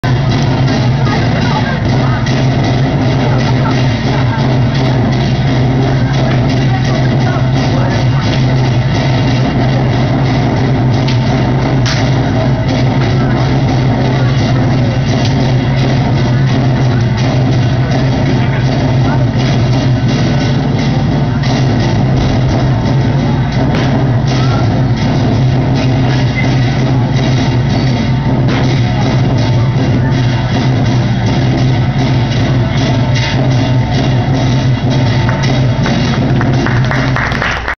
Sifu Lopez Shaolin Buddha Fist Tucson Meet Yourself
Sifu Lopez performs Shaolin Buddha Fist at Tucson Meet Yourself Video by Liam Lopez